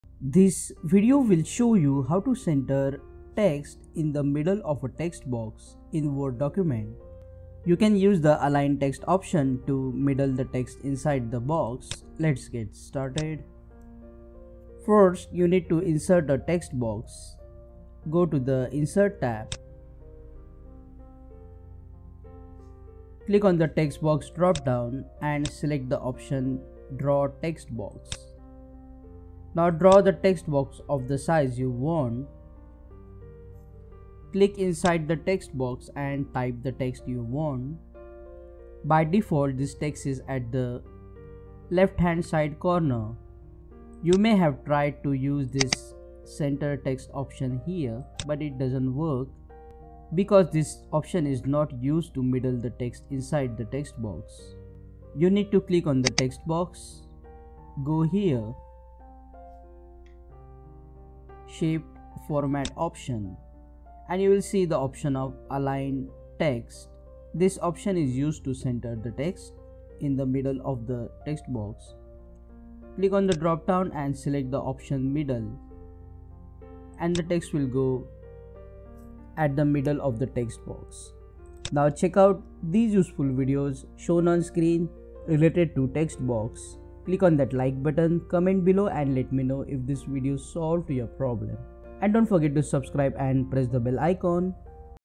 How To Center Text In The Middle Of a Text Box Word
Watch in this video How To Center Text In The Middle Of A Text Box Word document in Microsoft Word. To align text in middle of text box in Word use the aligh text option.